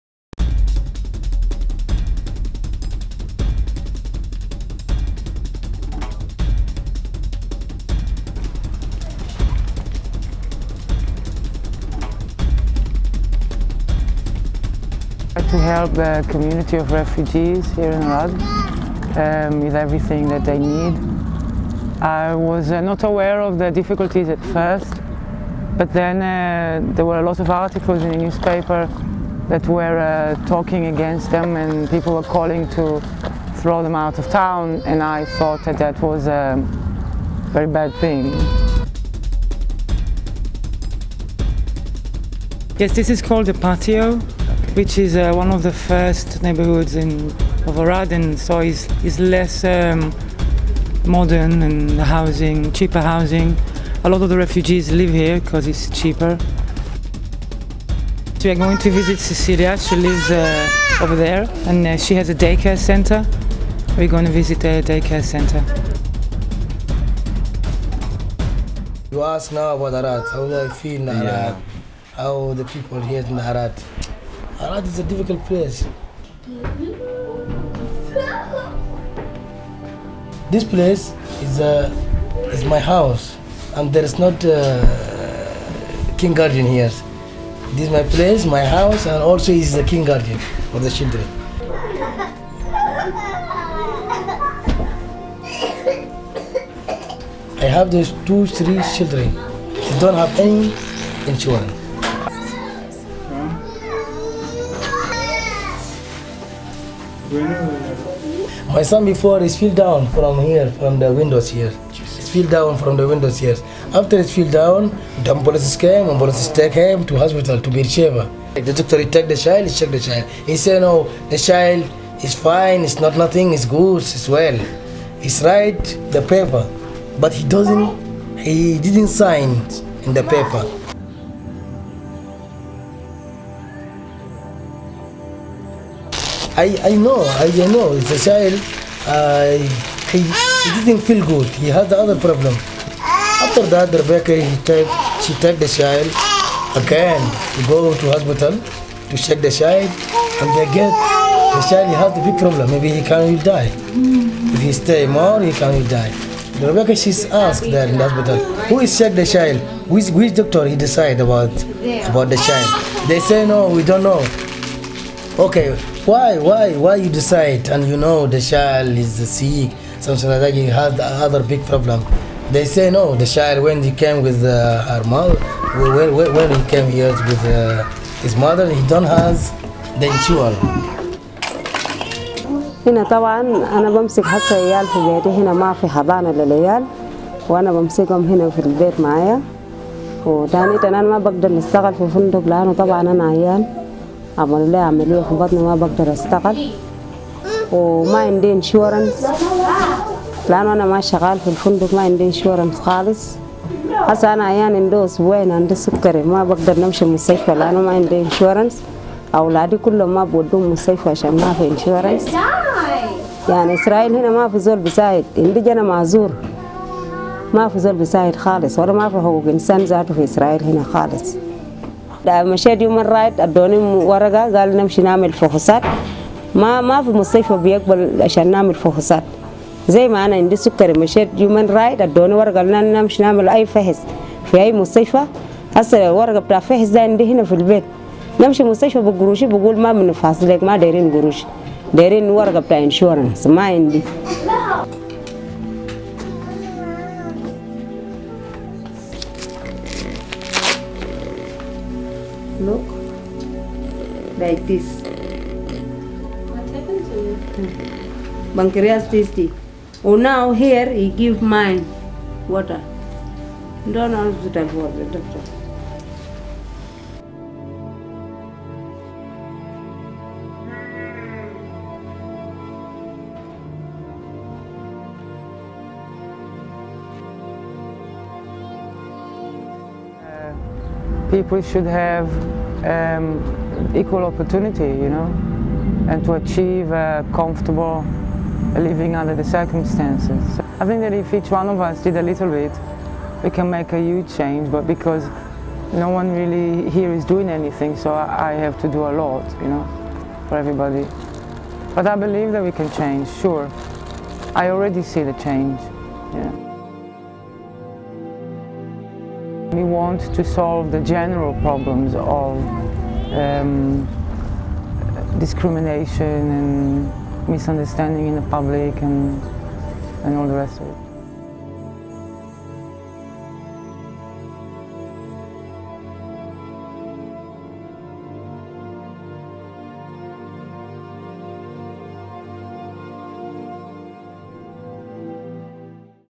Magic in the air
The stories of African refugees and asylum seekers in Arad